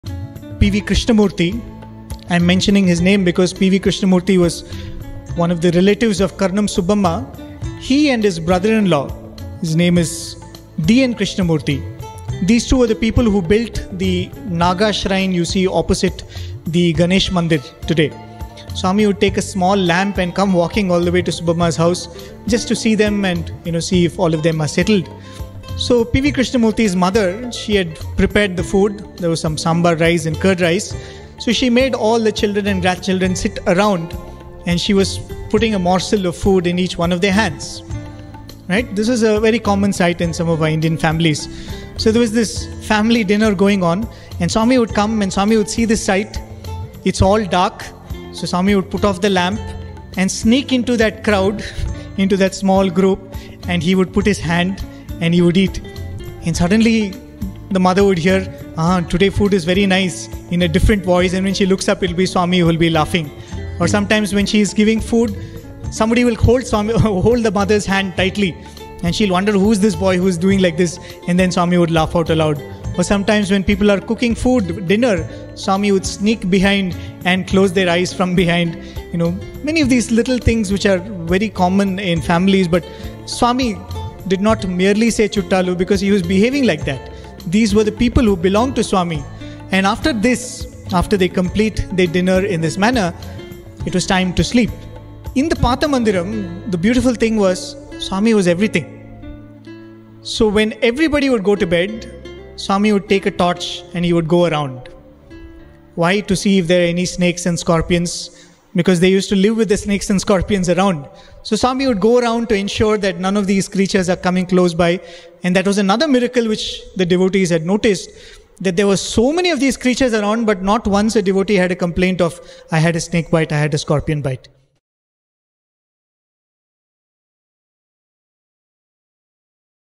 Dinnertime Pranks of Sai | Brother Prem Anosh
Brother Prem Anosh shares a heartwarming moment of our dearest Sai 🙂